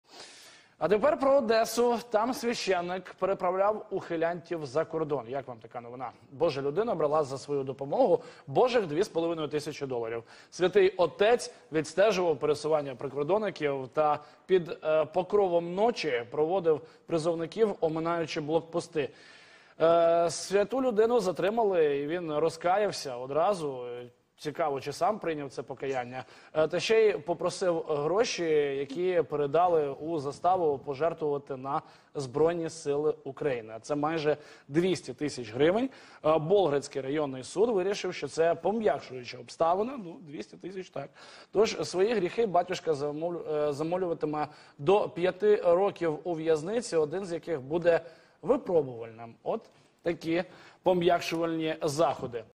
Священник з Одещини переправляв ухилянтів за кордон
За свої послуги він брав дві з половиною тисячі доларів.